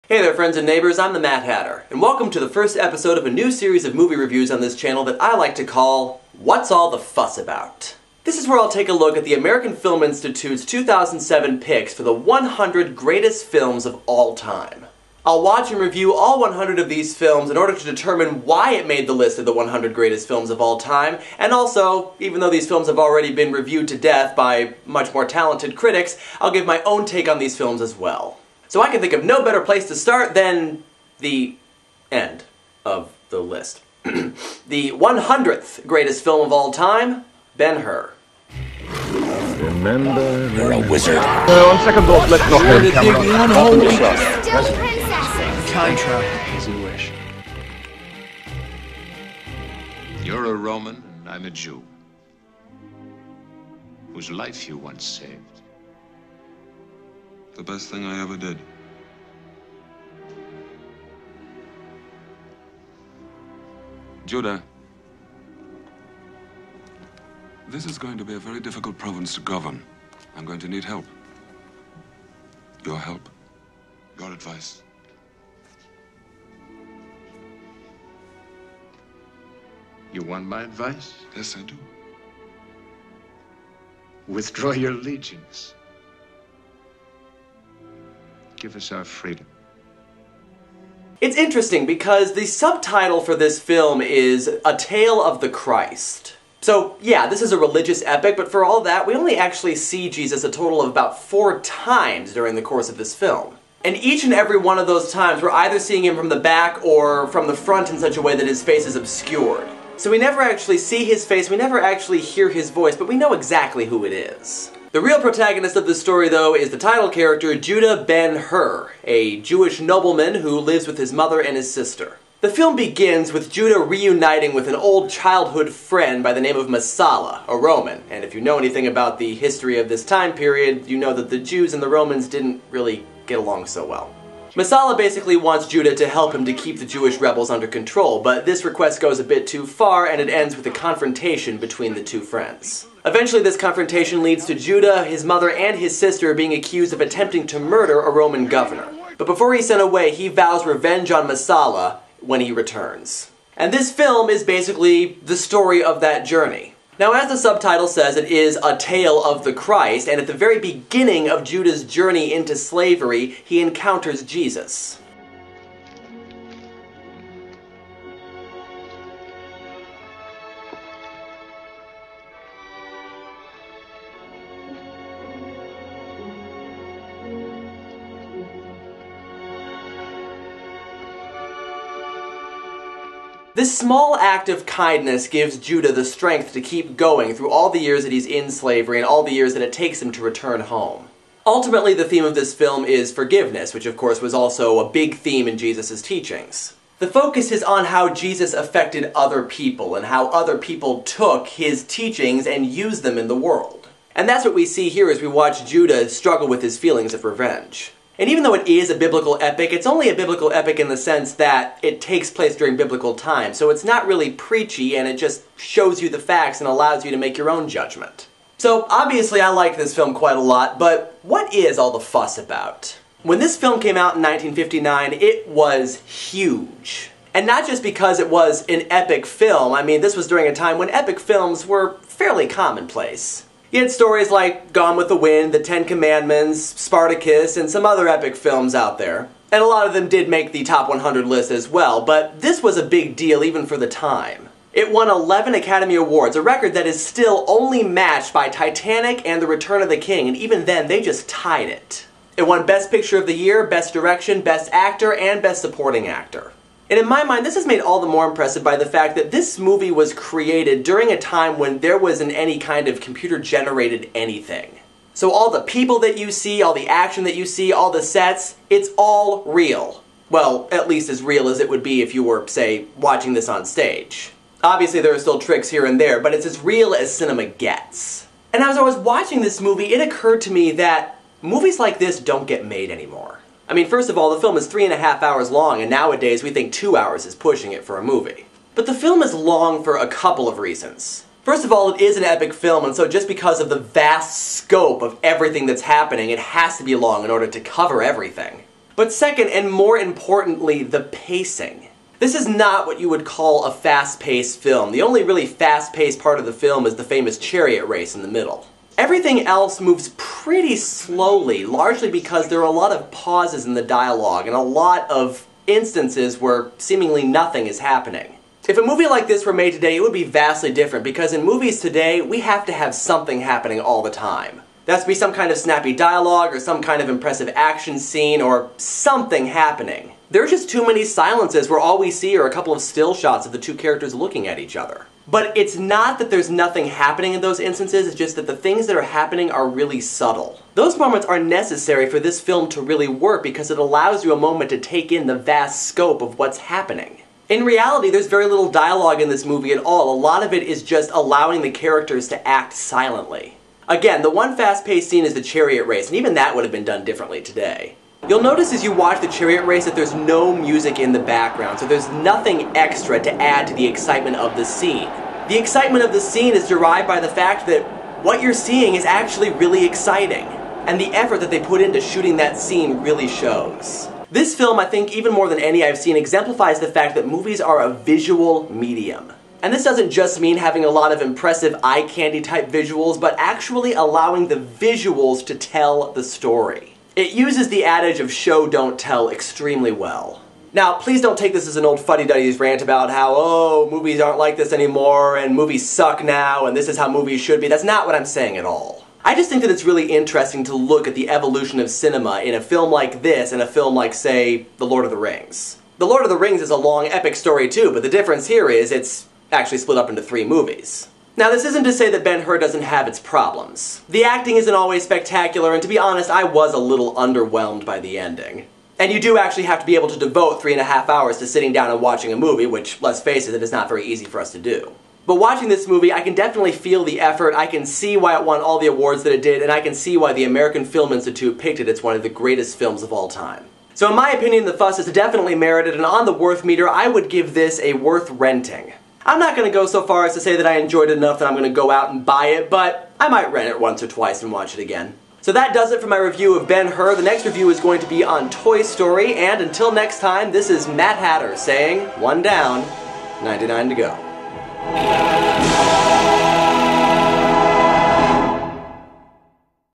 What's All The Fuss About? #100: Ben-Hur
In which Matt reviews AFI's #100 pick, Ben-Hur, in order to determine what all the fuss is about.

All clips owned by Warner Brothers.